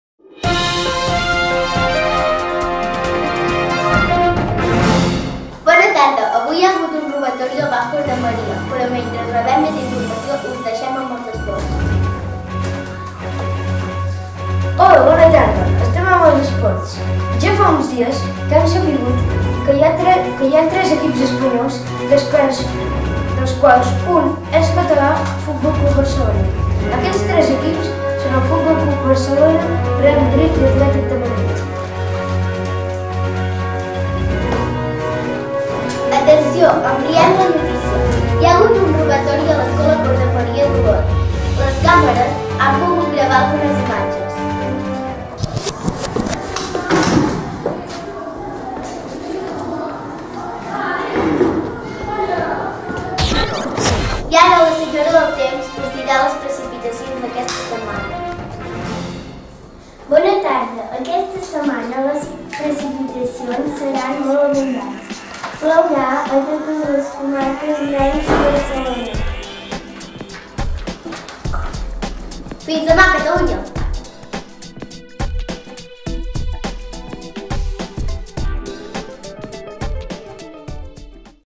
Telenoticies
Els alumnes de cicle superior hem realitzat un petit vídeo emmarcat dins del treball interdisciplinari: Vull treballar a la televisió!